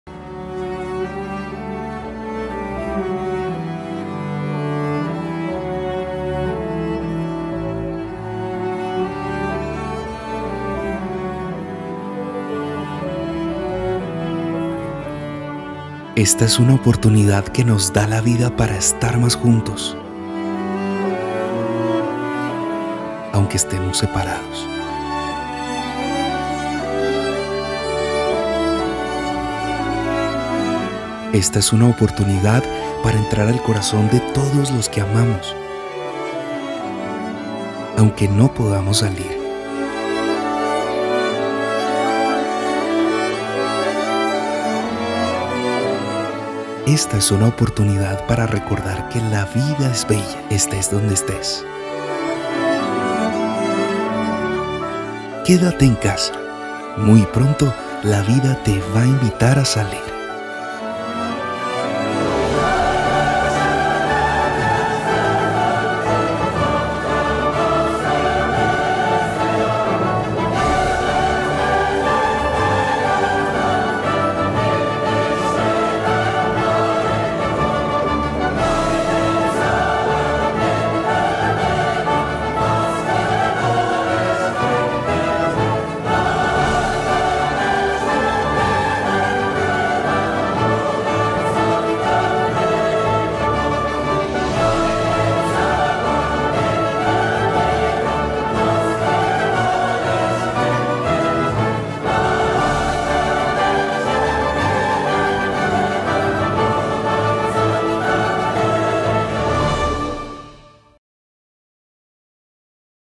Esta es una oportunidad que nos da la vida para estar más juntos.
Esta es una oportunidad que nos da la vida para estar más juntos, aunque estemos separados. Esta es una oportunidad para entrar al corazón de todos los que amamos, aunque no podamos salir. Esta es una oportunidad para recordar que la vida es bella, estes donde estés. Muy pronto la vida, te va invitar a salir.

Sigue al Ingeniero 👍 y QuédateEnCasa 🏡.